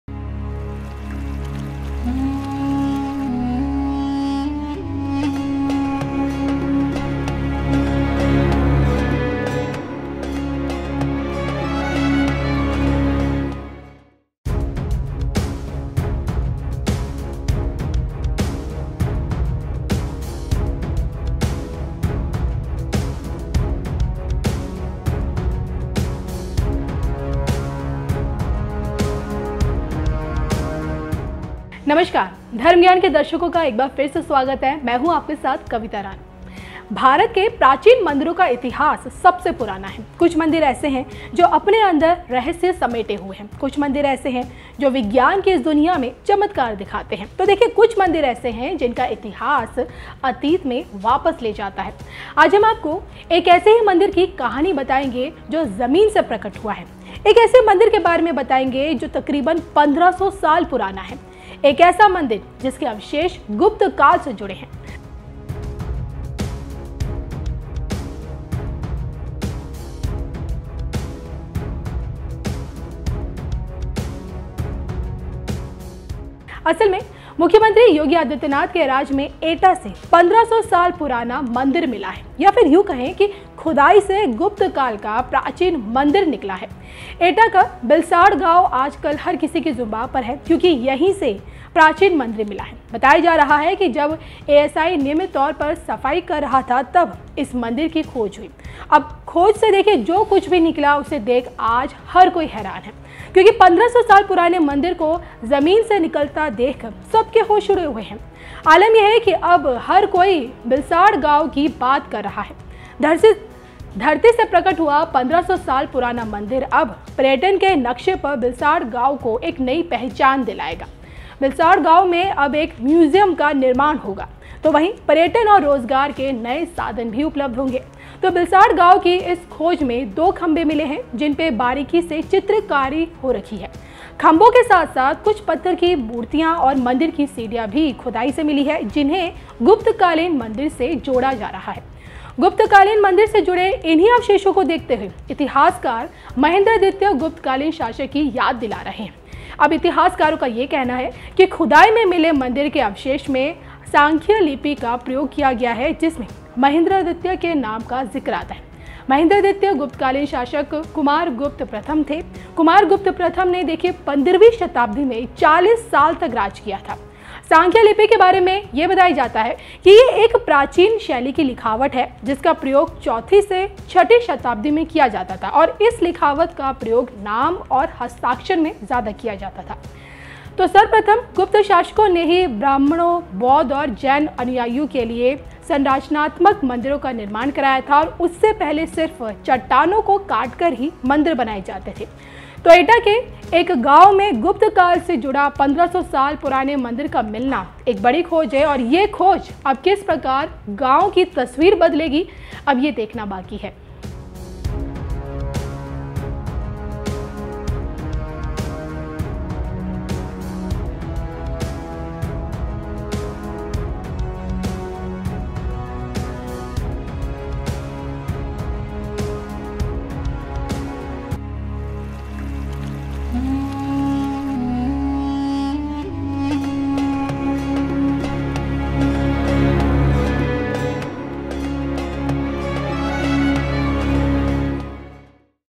Yogi राज में जमीन ने उगला 1500 साल पुराना राज़, सामने आया गुप्त काल का मंदिर
आज हम आपको एक ऐसे ही मंदिर की कहानी बताएंगे, जो जमीन से प्रकट हुआ है…एक ऐसे मंदिर के बारे में बताएंगे, जो तकरीबन 1500 साल पुराना है, एक ऐसा मंदिर जिसके अवशेष गुप्त काल से जुड़े हैं।..देखिये सिर्फ धर्म ज्ञान पर।